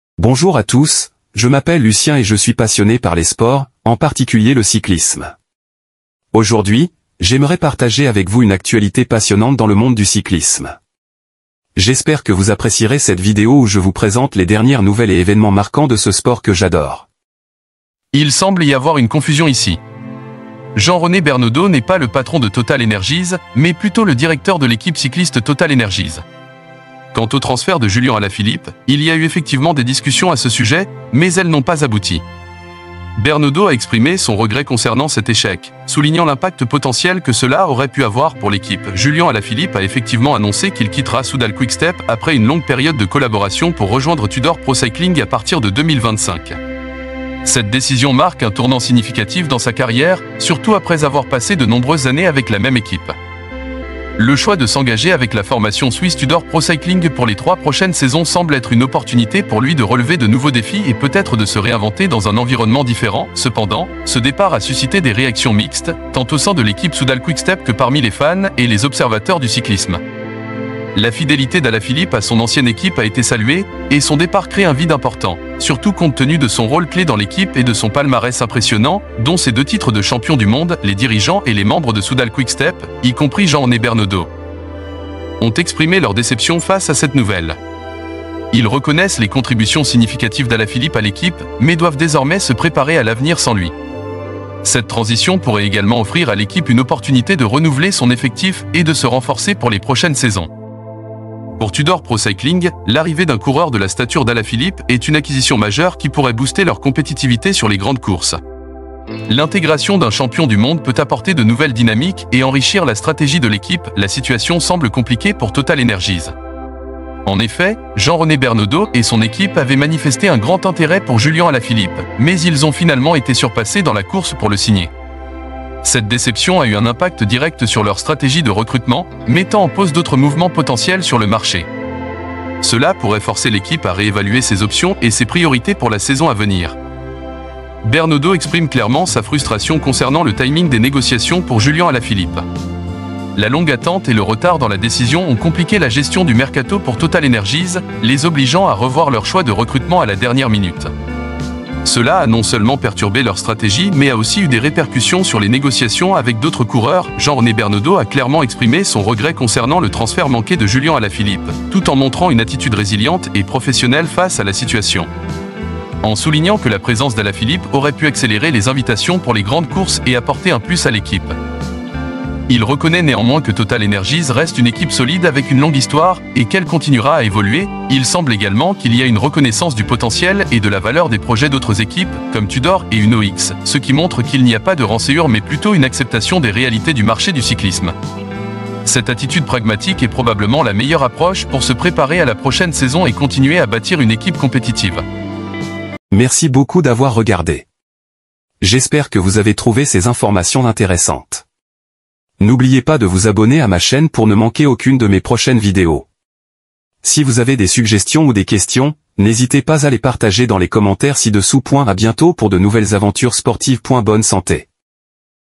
Alaphilippe, l’énorme coup dur
Julian Alaphilippe, 32 ans, quitte Soudal Quick-Step pour rejoindre Tudor Pro Cycling en 2025.
Total Energies, dirigée par Jean-René Bernaudeau, a été déçue par la décision tardive d'Alaphilippe après de longues négociations.
Bernaudeau regrette le timing des négociations mais reconnaît la solidité de son équipe pour la prochaine saison.